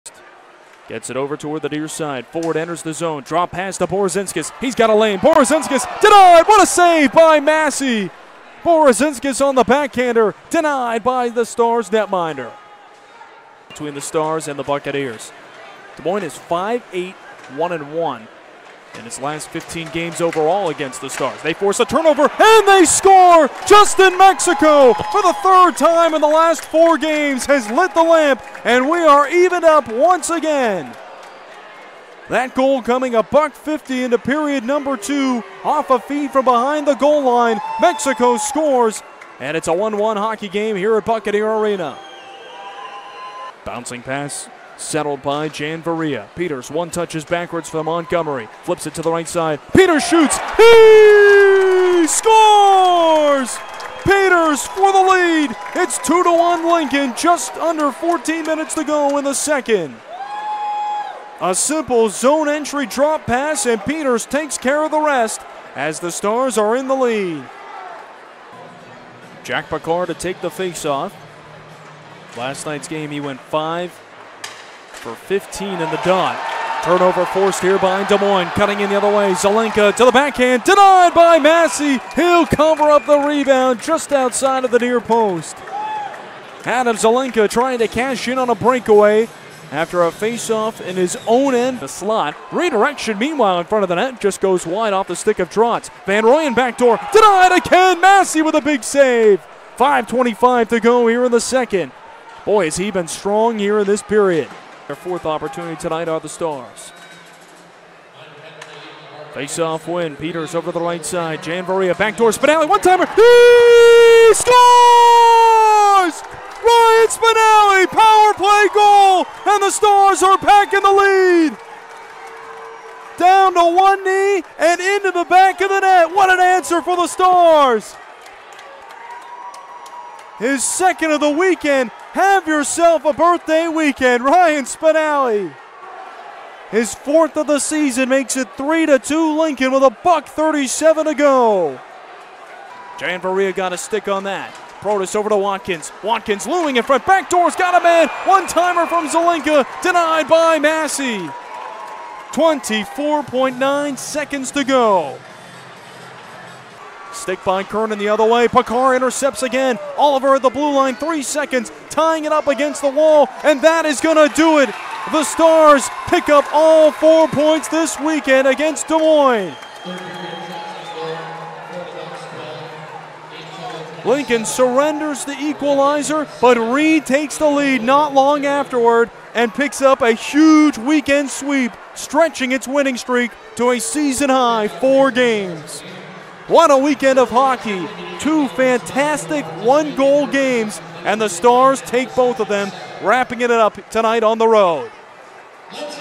Stars Highlights 2-10-24 (3-2 W @ Des Moines)
Justin Mexico, Keaton Peters and Ryan Spinale all scored while Lucas Massie earned his fifth consecutive win on Saturday night at Des Moines. Highlights are presented by Revolution Wraps.